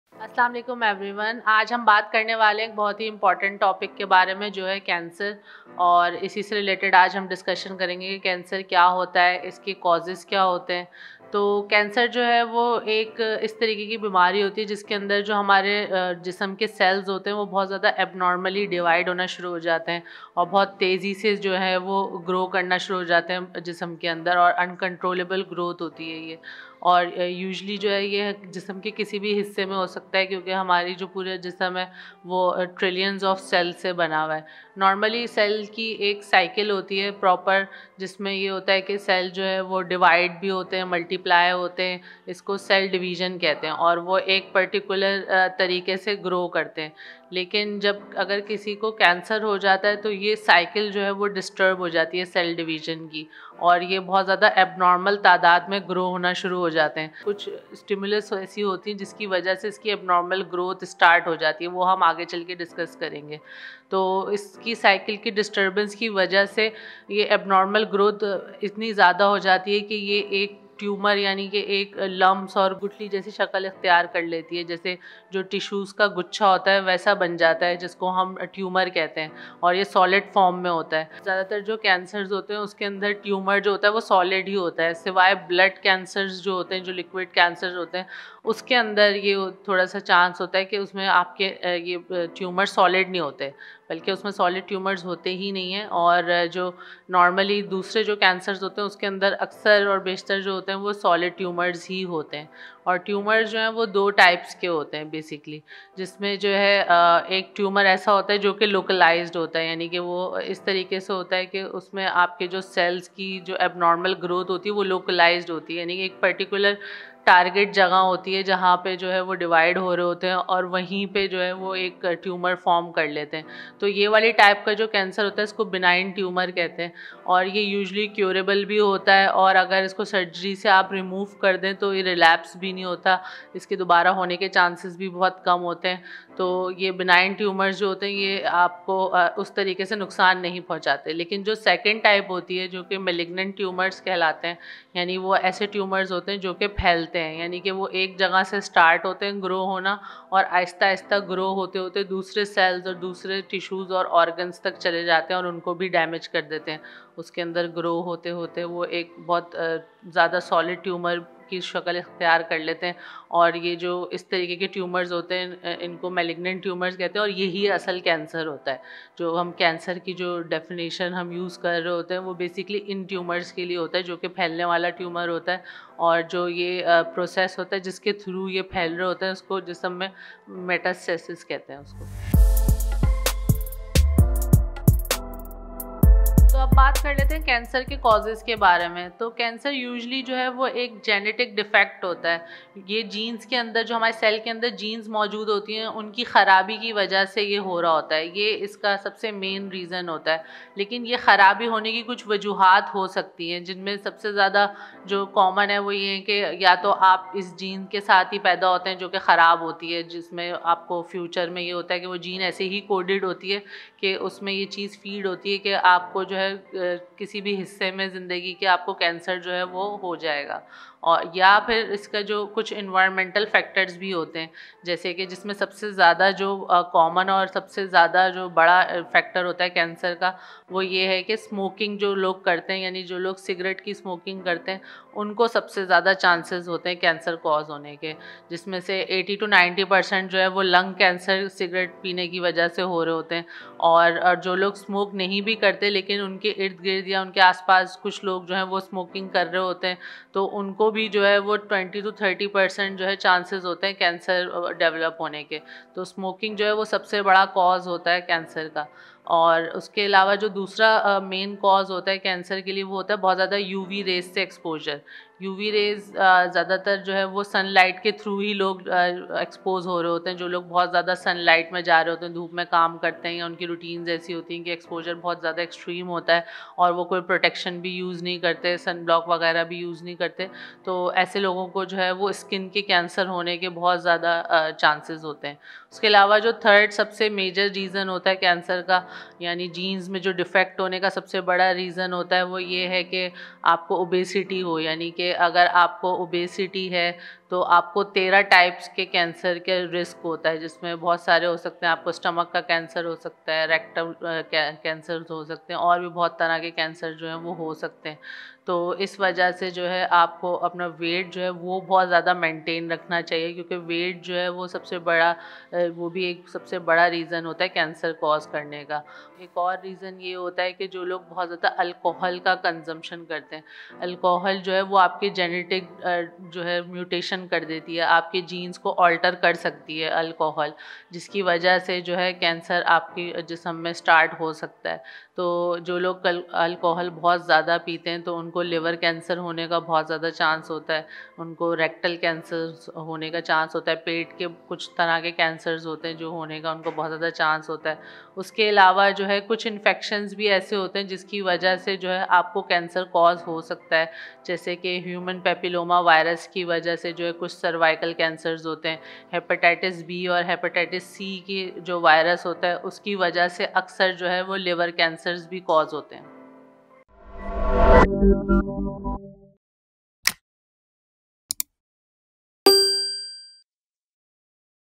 What is cancer? What causes cancer
Cancer is a disease in which some of the body's cells grow uncontrollably and spread to other parts of the body. Cancer can start almost anywhere in the human body, which is made up of trillions of cells.

Causes of Cancer?
Cancer is a genetic defect, It happens due to the disfunction of the genes present in our cells. And some of the environmental factors are: Tobacco use, alcohol consumption, unhealthy diet, physical inactivity and air pollution

00:00 What is Cancer?
03:03 Causes of Cancer?